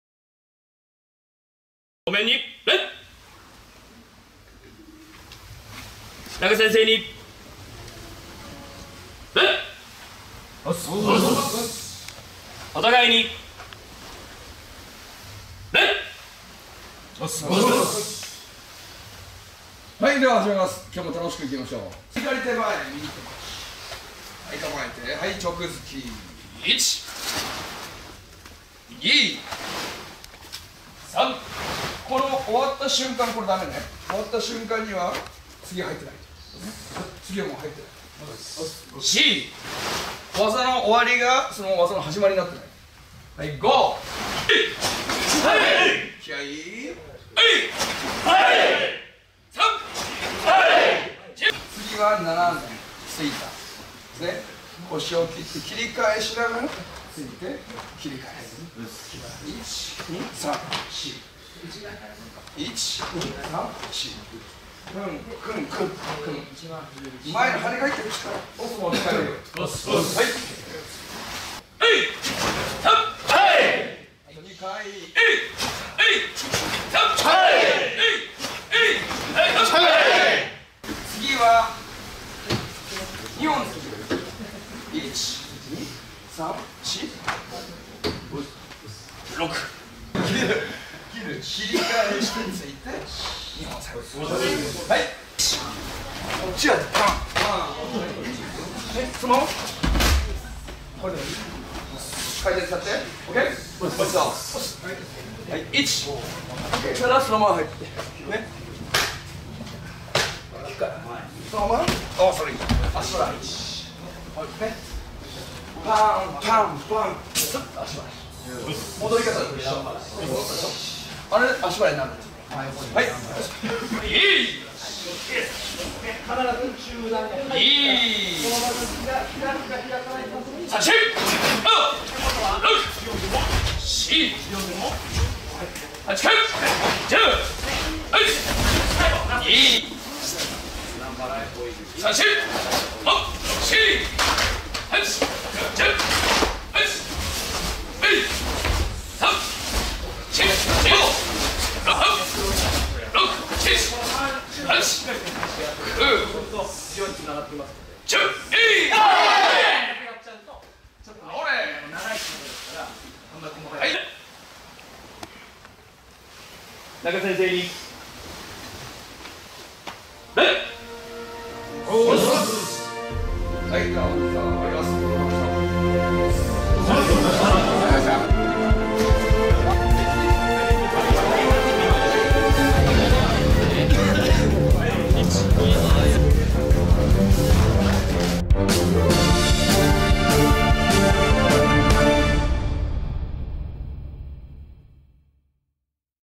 Tatsuya Naka Shihan’s Karate Dojo (Taishi-juku)
Naka shihan's Dojo "Taishi-juku" Do you want to train Karate together?

★KURO-OBI WORLD INTERNATIONAL SERVICE
Streaming Subscription with English subtitles